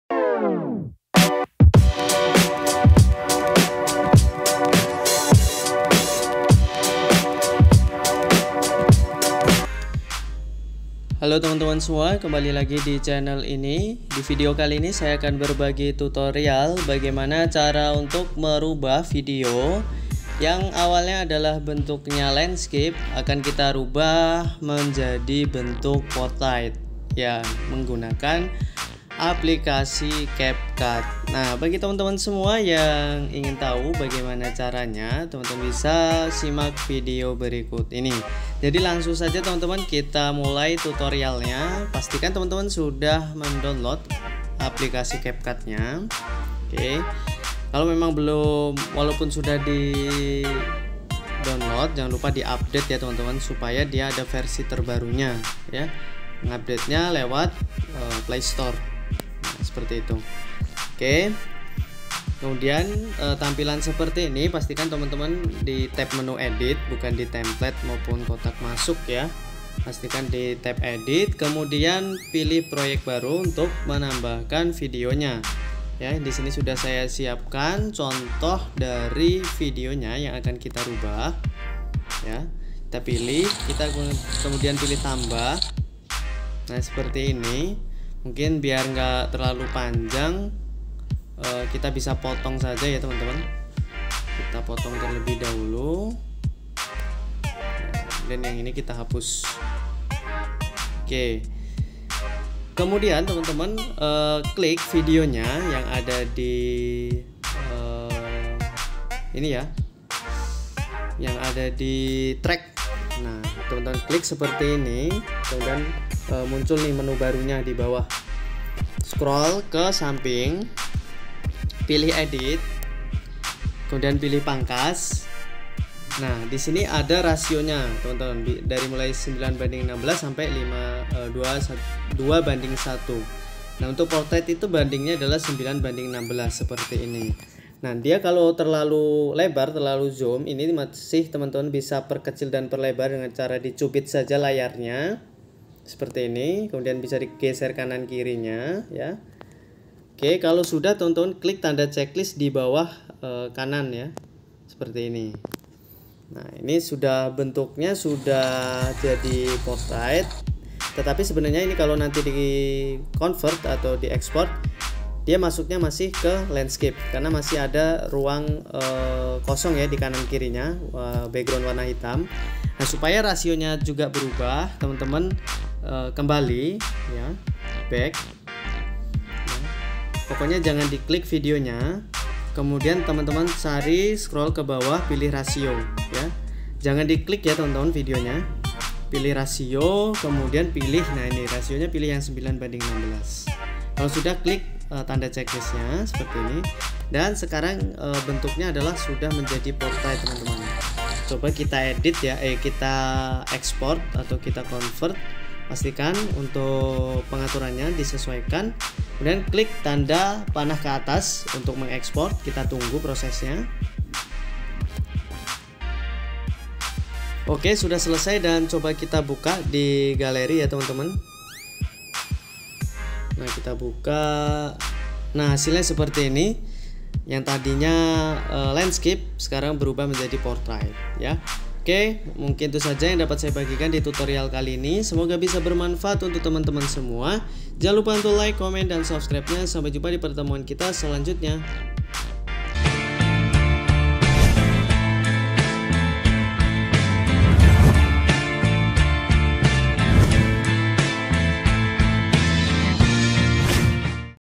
Cara Merubah Video Landscape Jadi Portrait Aplikasi Capcut
Cara Mengubah Video Potrait Menjadi Landscape Di Capcut
Kali ini saya akan membagikan tutorial ubah video landscape jadi portrait. Untuk mengubah video landscape ke vertikal ini saya menggunakan aplikasi capcut, aplikasinya bisa kalian download di playstore.

Dukungan dari kalian sangat bermanfaat bagi saya